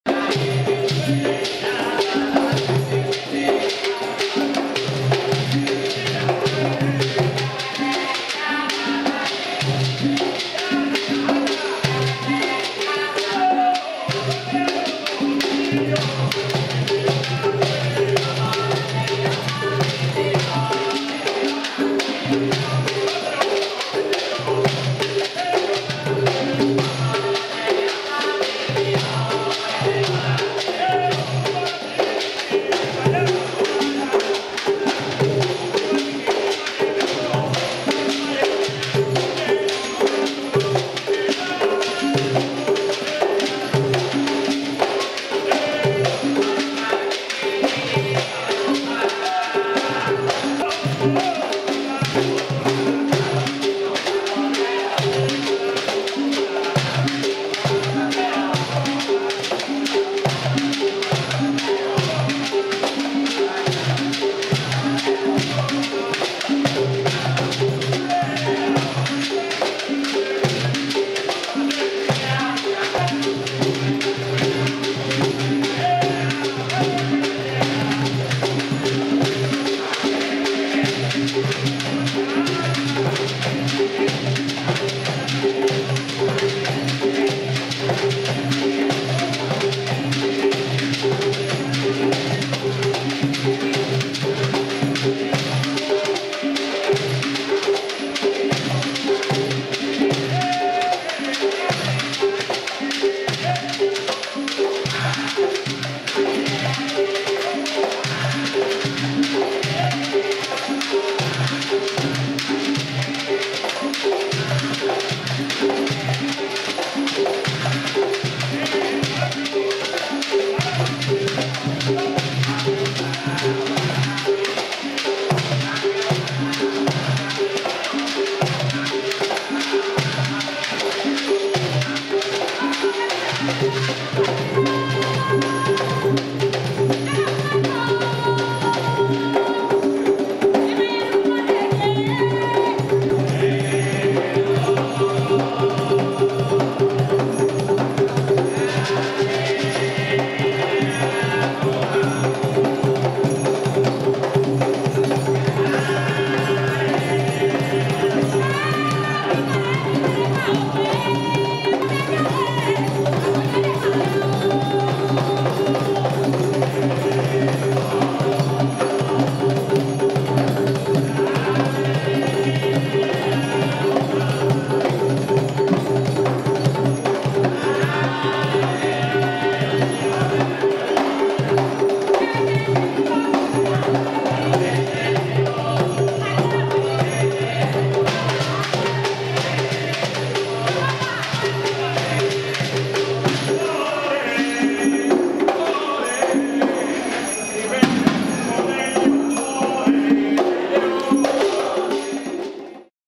grupo ibeji - yemaya
Grupo Ibeji  dance for Yemaya, performed Havana, Cuba. Original video filmed / edited by Tim Harrison, Havana, Cuba